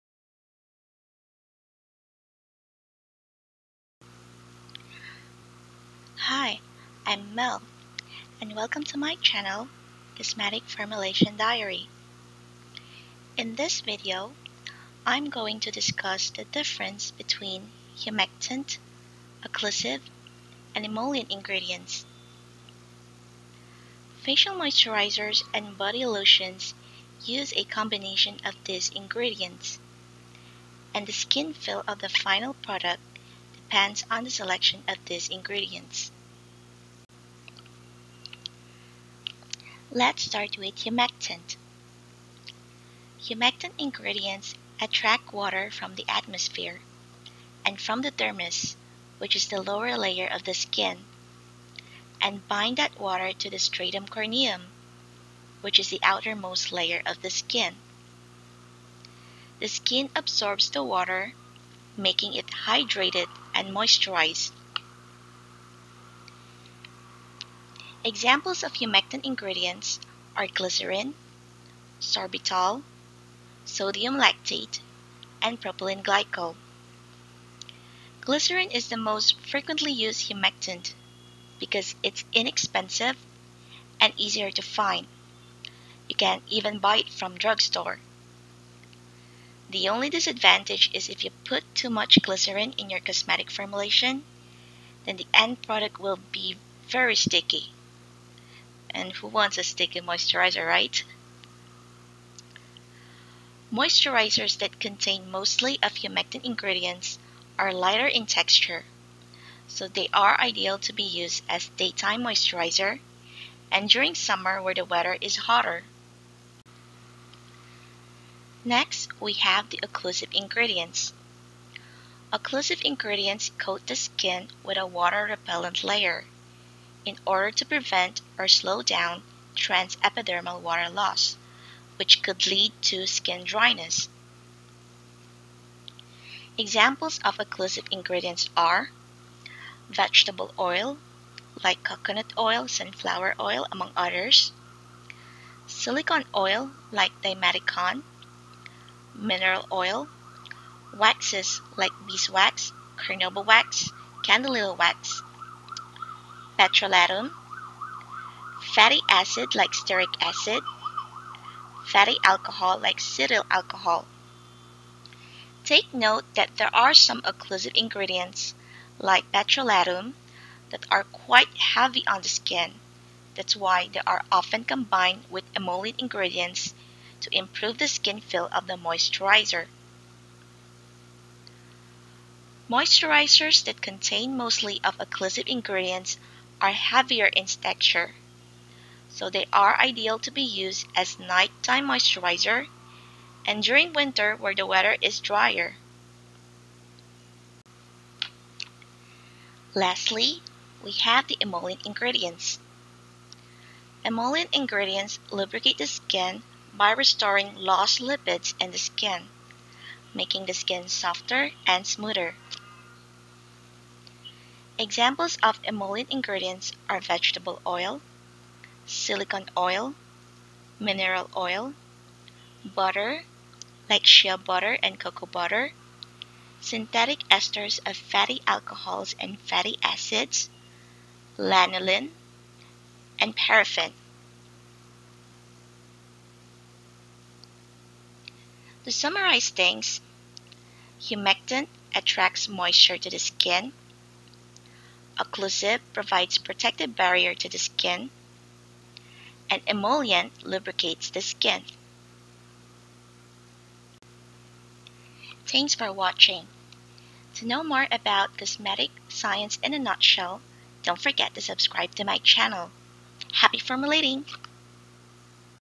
Cosmetic Formulation - Humectant, Occlusive and Emollient
In this video, I discuss the difference between humectant, occlusive and emollient ingredients. Moisturizers use a combination of these ingredients and the final feel of the product depends on which ingredients you have selected.